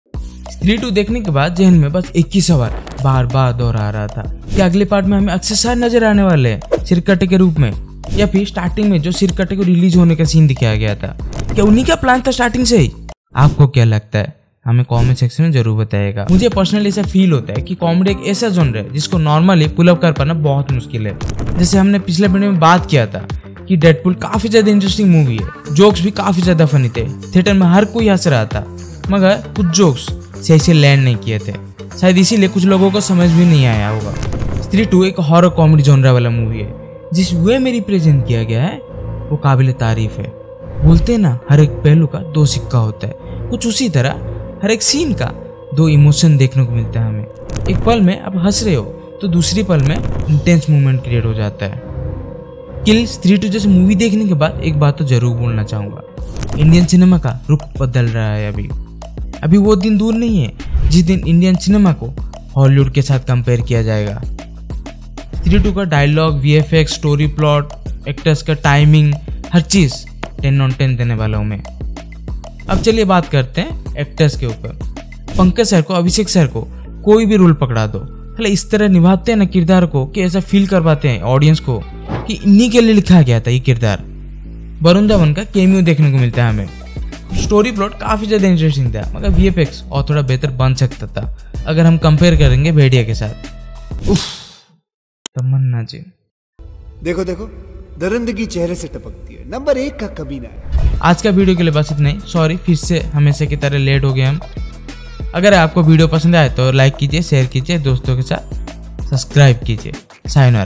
Stree 3 Movie Spoiler 😱! Stree 2 Full Movie Review | It Review Time
In this video, we have explained the details about Stree 2 Full Movie. Let us know what is your take on this video.